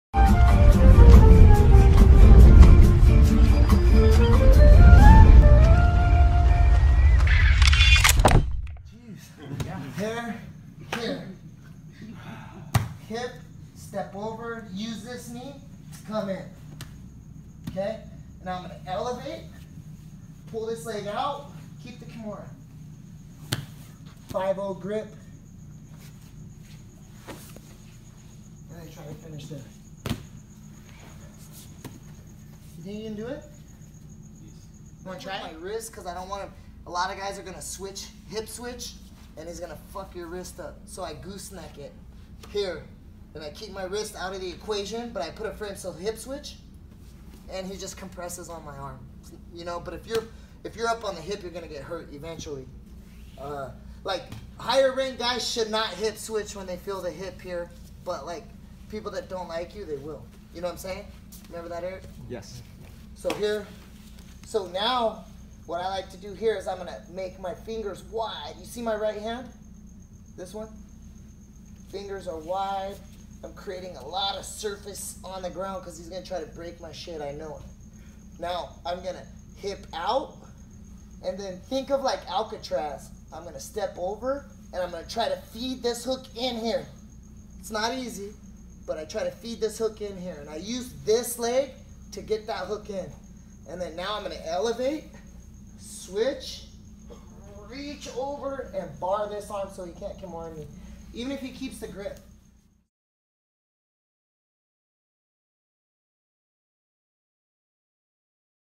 Kimura counter
I wasn't aware that this was being filmed.  This is a counter to the Kimura that I like to use.  Make sure to control the arm in danger by keeping it firmly on the ground.